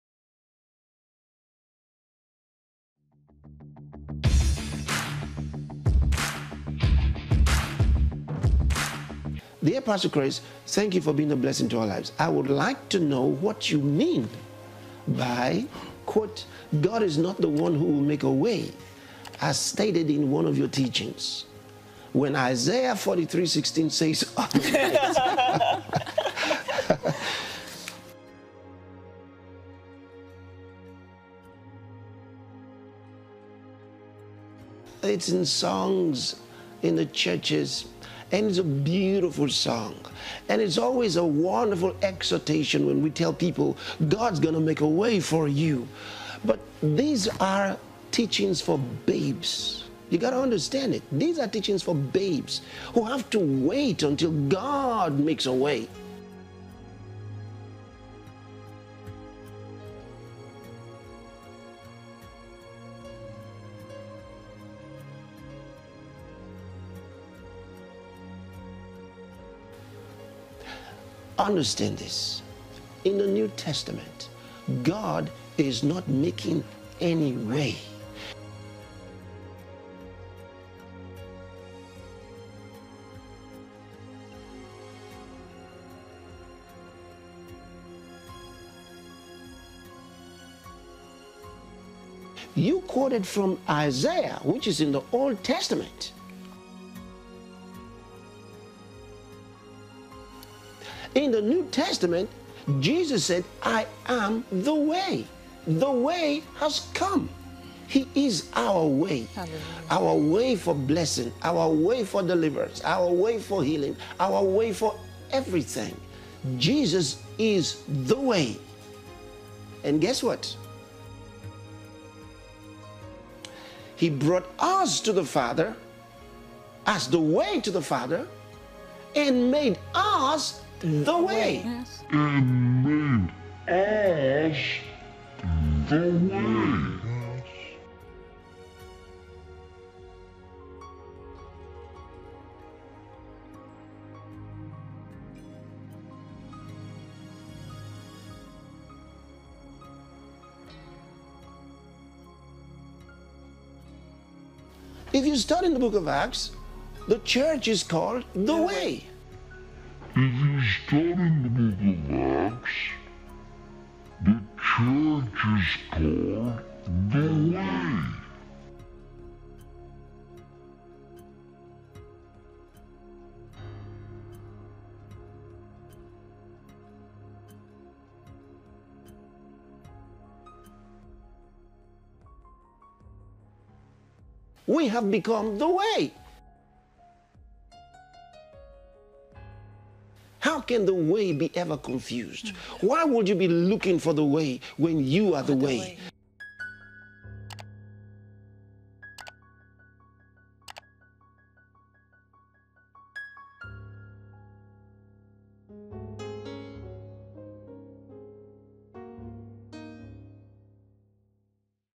'I am the way' Pastor Chris Oyakhilome
The assertion put forth by Pastor Chris Oyakhilome, positing that the church has supplanted Jesus as the exclusive means to salvation (Being the way), merits critical examination within the framework of prevailing Christian doctrines. Hence this short video.